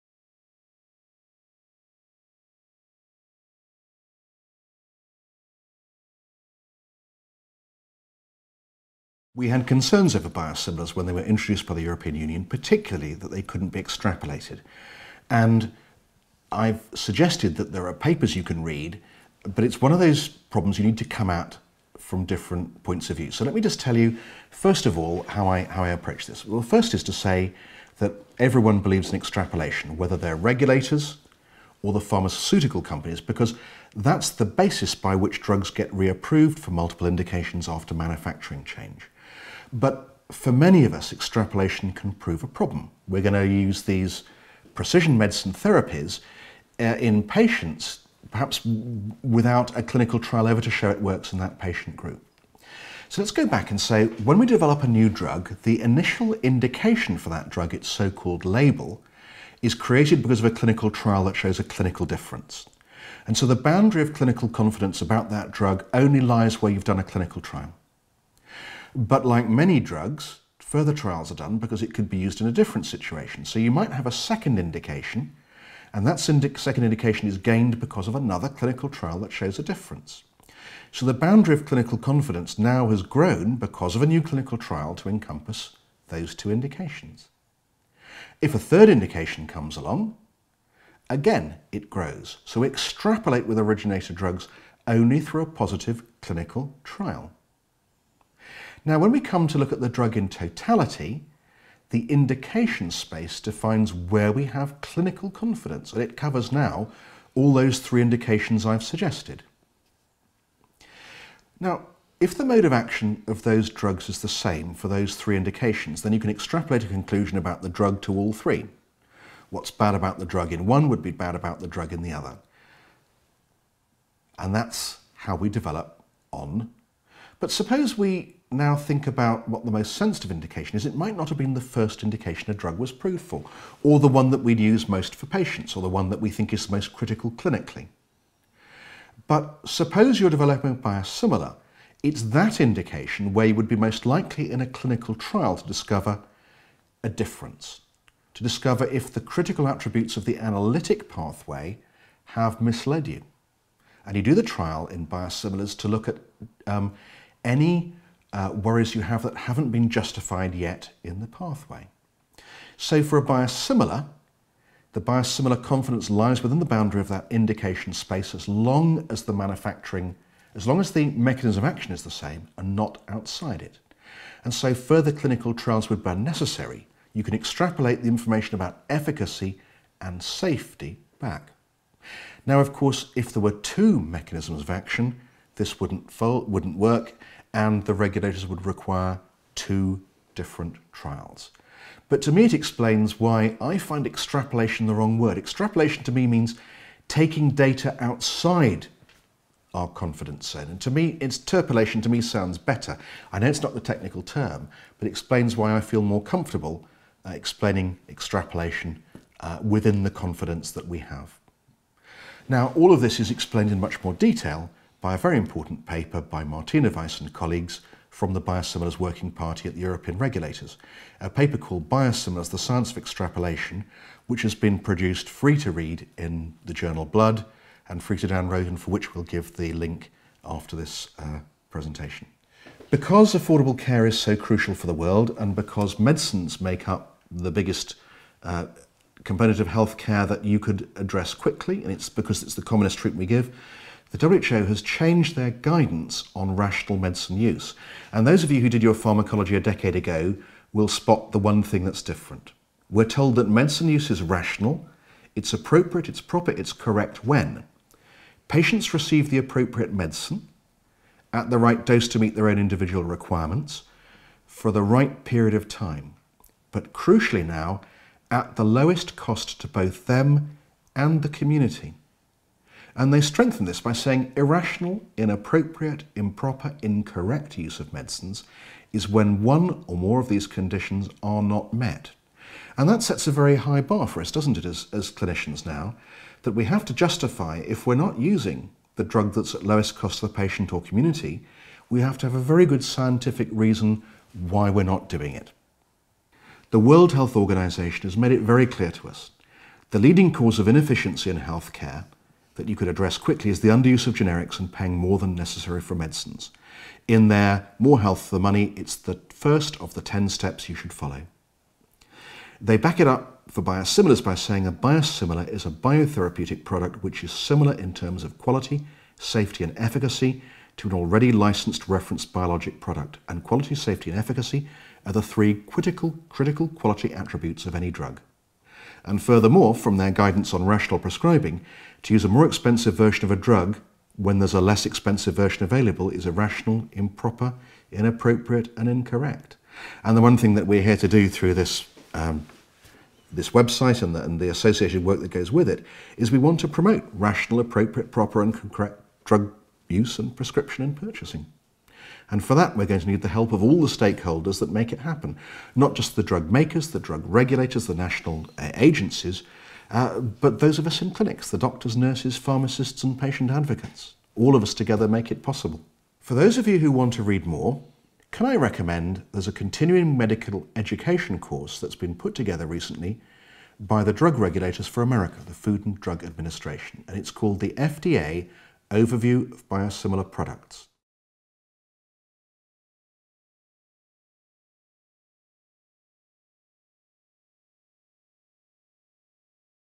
Biosimilars: extrapolation & rational medicine use
Paul Cornes, MD from the University Hospital Bristol NHS Foundation Trust, Bristol, UK talks about the rationale behind extrapolation of indication for biosimilars.